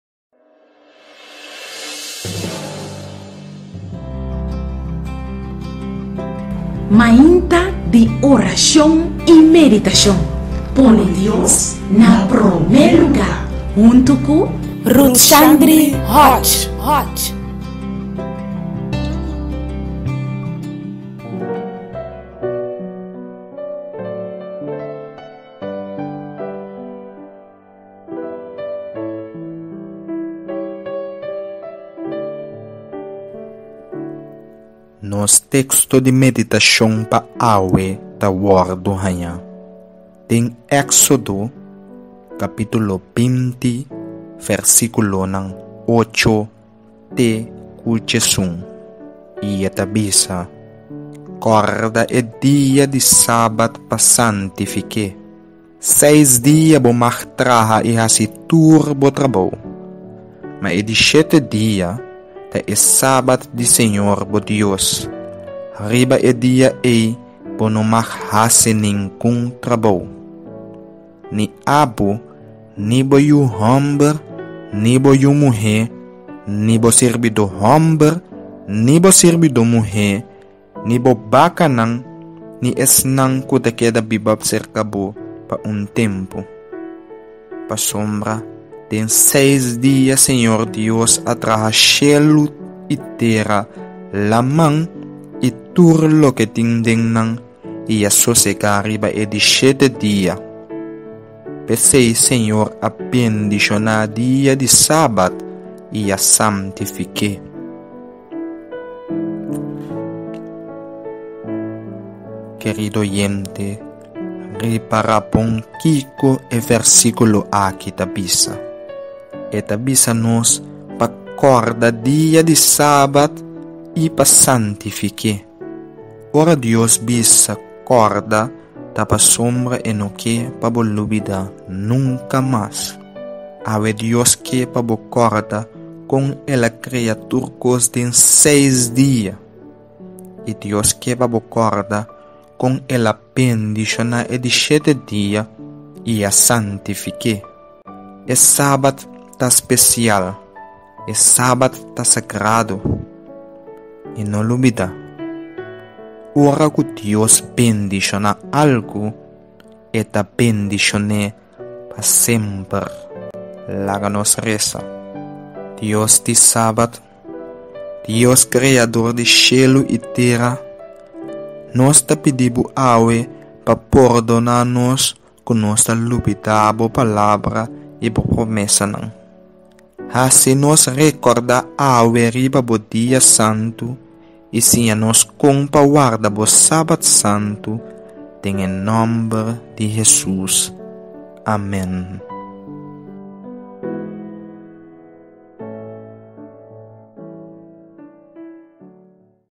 KÒRDA e BENDISHON / Mainta di Orashon Orashon i Meditashon/ 08-09-2017/ Rudchandry Hodge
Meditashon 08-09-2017

KÒRDA e Dia Bendishoná!!

DIOS Kreador,
UN biaha mas mi ta Bini den Bo presensia ku gradisimentu. Pa yamaBo danki pa TUR e BENDISHONNAN di awe. Danki pa Bo dia tan spesial ku Bo a bendishoná mi ku ne. Siñami mas di Bo dia Santu i siñami Kon pa warda e dia Aki Komo UN dia Santu, Sagrado i Apart for di TUR otro dia di kreashon. Bendishoná mi awe. Dunami un fin di siman bendishoná i apartá pa un propósito spesial serka Bo ooh mi Dios. TA mi Orashon, den e nòmber di Hesus. AMÈN i AMÈN i AMÈN i AMÈN.

DIOS bendishoná bo i UN felis SABAT